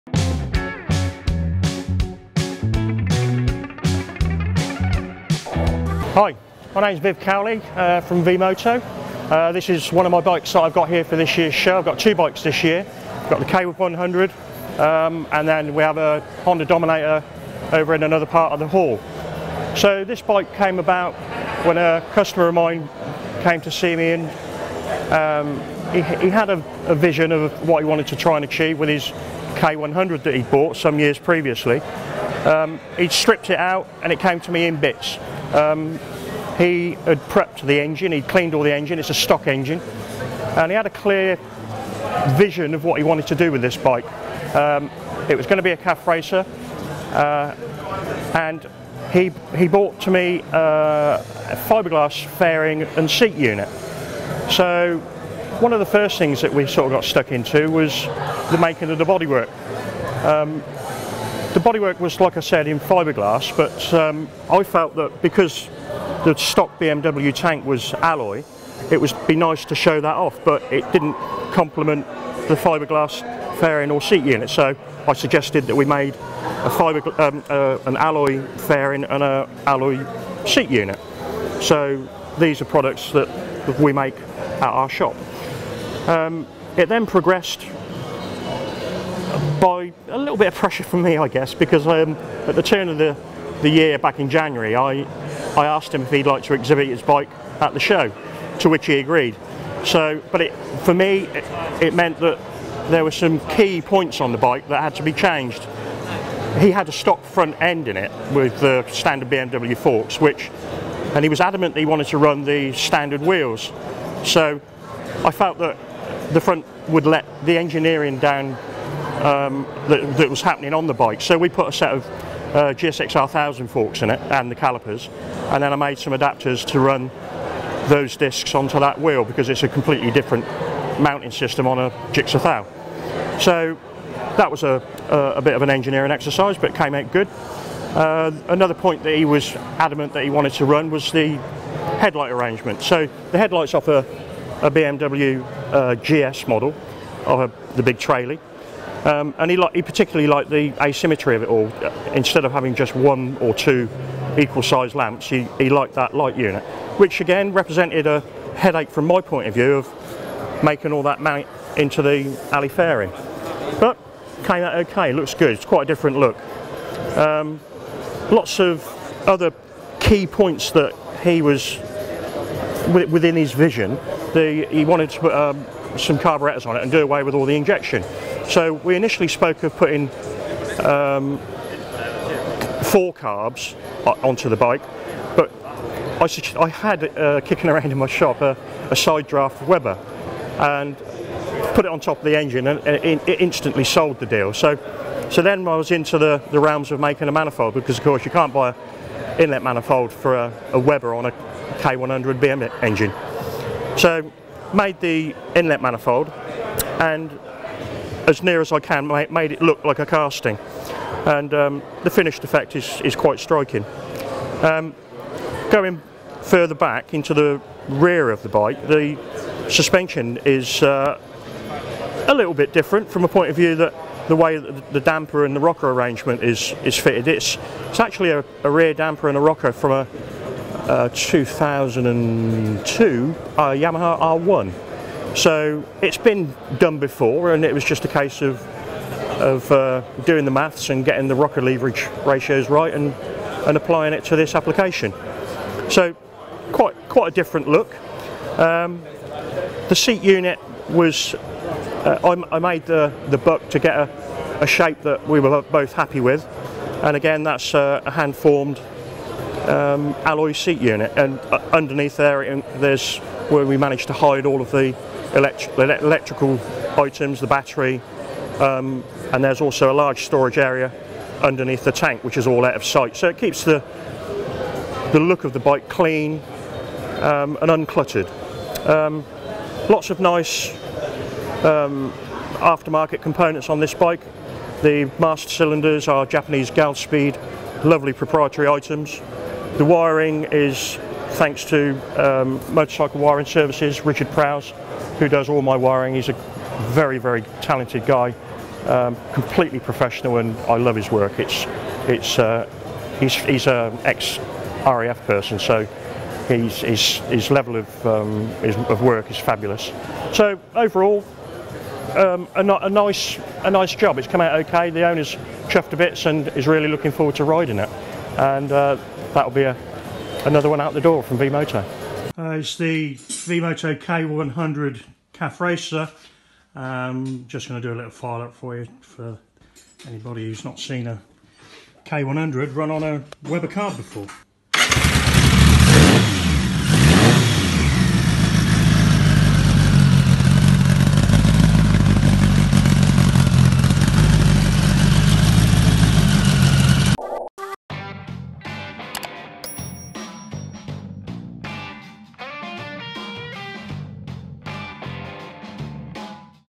V-MOTO: BMW K100 Cafe Racer - Bike Shed Show 2019
Viv Cowley from V-Moto has been featured on The Bike Shed many times before. For our Bike Shed Show 2019 Viv brought two bikes, here's the second, a cafe racer build based on a BMW K100

For those of you who have seen Viv's bikes before, you'll already know that his attention to detail and craftsmanship is second to none. Don't forget to wait to the end to hear it start up.